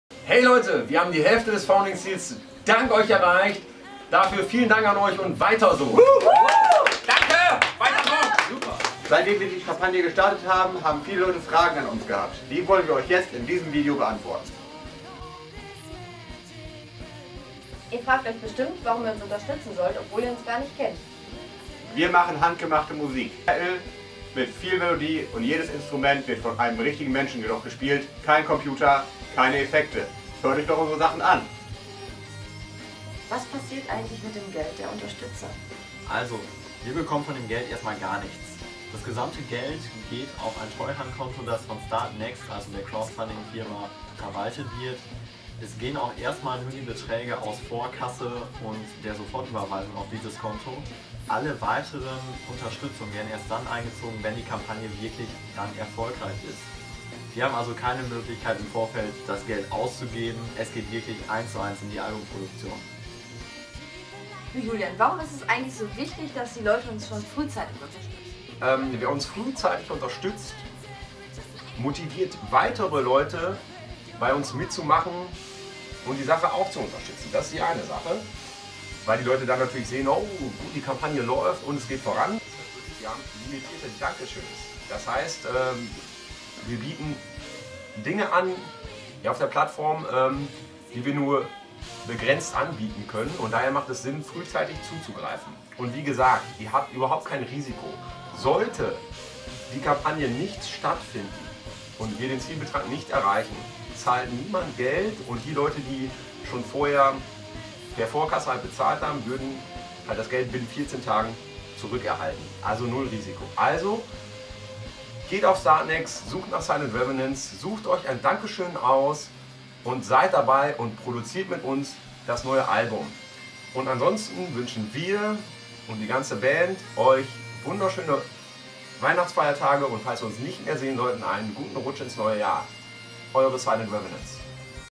Silent Revenants Crowdfunding Kampagne -Update-
Hey Folks,

Die Crowdfunding Kampagne zu unserem Debütalbum geht mit großen Schritten voran. Wir haben bereits knapp die Hälfte geschafft, Zeit für ein kleines Update!!

Während der esten Tage sind uns viele Fragen zur Kampagne gestellt worden. WIr wollen dieses Videoupdate nutzen, diese zu beantworten.